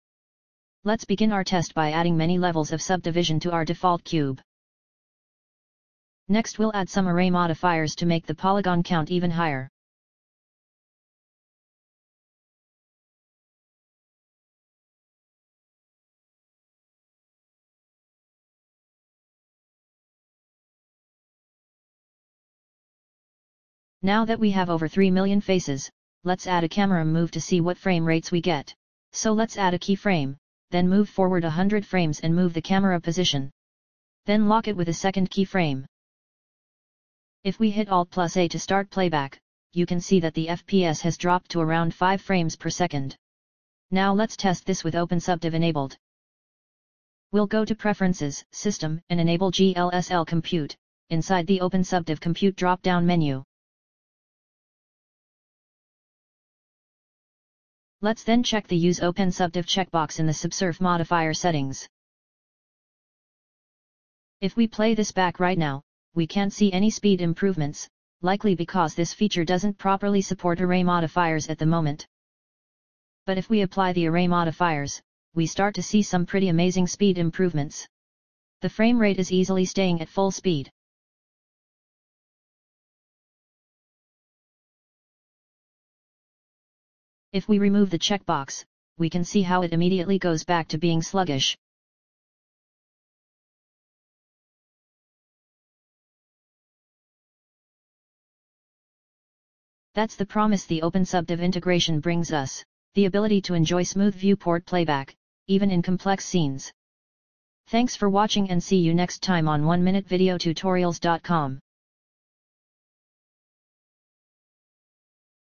Blender 2.76 Open Subdiv test
In this video we use the new Open Subdiv integration to improve viewport playback performance in a complex Blender scene.